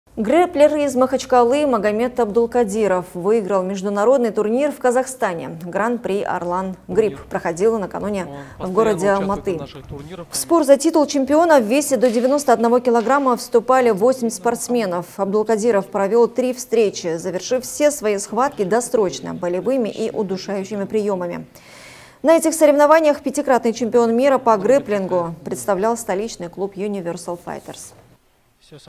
Магомед Абдулкадиров стал чемпионом международного турнира Arlan Grip PRO
Грэпплер из Махачкалы Магомед Абдулкадиров выиграл международный турнир в Казахстане. Гран-при Arlan Grip проходило накануне в городе Алматы.

В спор за титул чемпиона в весе до 91 кг вступали 8 спортсменов. Абдулкадиров провел три встречи, завершив все свои схватки досрочно – болевыми и удушающими приемами. На этих соревнованиях пятикратный чемпион мира по грэпплингу представлял столичный клуб Universal Fighters.